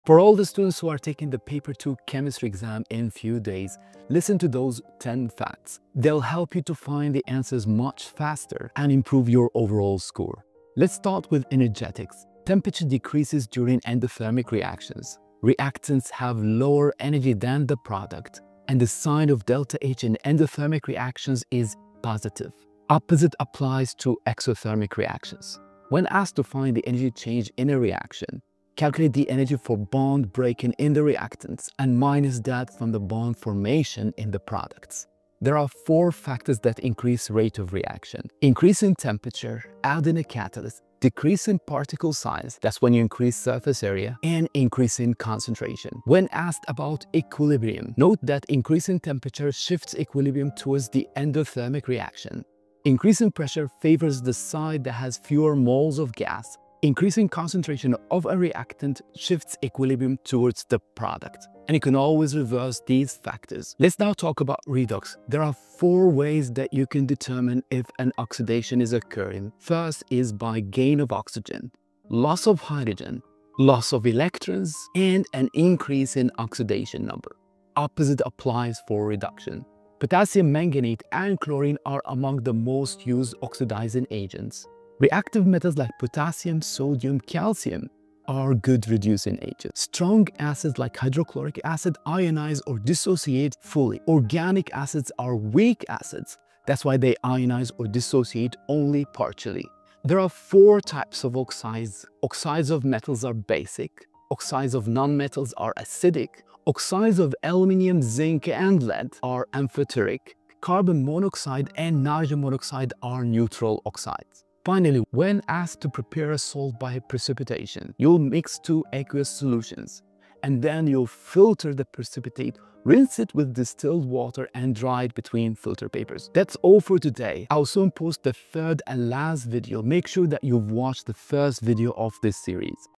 20 IGCSE chemistry paper 2 predicted questions for 2026 exams (part 2)
Get the full course of IGCSE Chemistry and study by your own pace 👇

IGCSE chemistry paper 2 exam hacks part 2
 • 📚 For all the students taking the Paper 2 Chemistry exam soon, listen to these 10 facts to find answers faster and improve your score.
 • 🔥 Energetics:
 • 🌡️ Temperature decreases during endothermic reactions.
 • ⚡ Reactants have lower energy than the product in endothermic reactions.
 • ➕ The sign of ∆ H in endothermic reactions is positive; the opposite applies to exothermic reactions.
 • 🔍 To find the energy change in a reaction, calculate the energy for bond breaking in the reactants and subtract that from the bond formation in the products.
 • ⏱️ Factors Increasing Rate of Reaction:
 • 🔺 Increasing temperature.
 • ⚗️ Adding a catalyst.
 • 📏 Decreasing particle size (increases surface area).
 • 💧 Increasing concentration.
 • ⚖️ Equilibrium:
 • 🌡️ Increasing temperature shifts equilibrium towards the endothermic reaction.
 • 📈 Increasing pressure favors the side with fewer moles of gas.
 • 🧪 Increasing concentration of a reactant shifts equilibrium towards the product.
 • 🔄 These factors can be reversed.
 • 🔄 Redox Reactions:
 • 🔍 Determine oxidation by:
 ▪ ➕ Gain of oxygen.
 ▪ ➖ Loss of hydrogen.
 ▪ ⚡ Loss of electrons.
 ▪ 🔢 Increase in oxidation number.
 • 🔄 Opposite applies for reduction.
 • 🟣 Potassium manganate and chlorine are common oxidizing agents.
 • ⚙️ Reactive metals like potassium, sodium, and calcium are good reducing agents.
 • 🧪 Acids:
 • 💪 Strong acids like hydrochloric acid ionize or dissociate fully.
 • 🍋 Organic acids are weak acids and ionize or dissociate only partially.
 • 🧱 Types of Oxides:
 • 🧱 Oxides of metals are basic.
 • 🌿 Oxides of nonmetals are acidic.
 • ⚖️ Oxides of aluminium, zinc, and lead are amphoteric.
 • ⚪ Carbon monoxide and nitrogen monoxide are neutral oxides.
 • 🧂 Salt Preparation by Precipitation:
 • 🧪 Mix two aqueous solutions.
 • 🧼 Filter the precipitate, rinse with distilled water, and dry between filter papers.